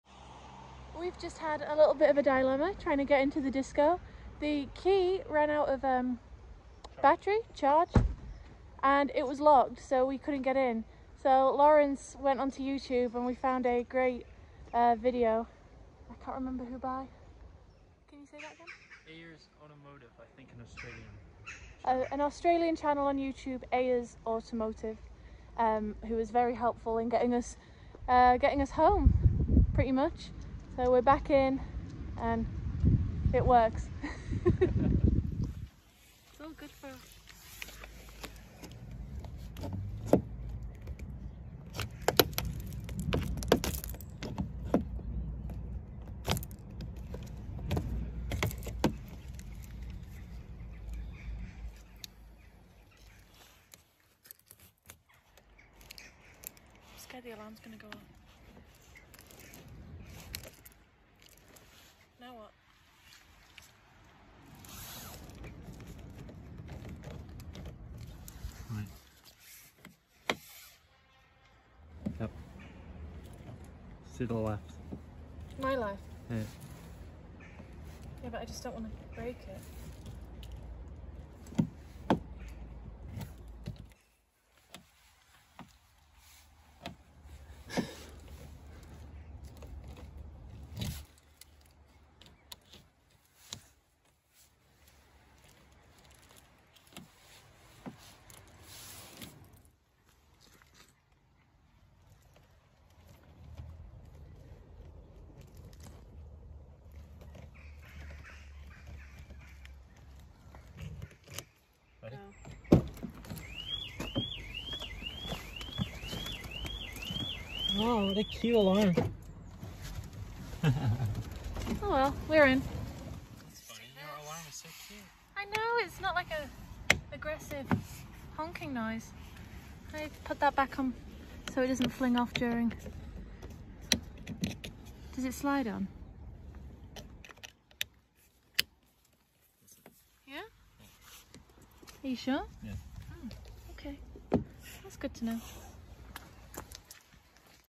The One Where We Get Locked Out of the Land Rover Discovery 3
It’s not Monday without a little bit of manic, but just a little. We’ve been waiting for the day the key might run out of charger (since accidentally going through our washing machine).

We were only locked out for a couple of minutes, after finding a rather easy troubleshooting video via Ayers Automotive Solutions on YouTube (link below).

There’s probably a few other ways of doing this, but this seemed like the first option.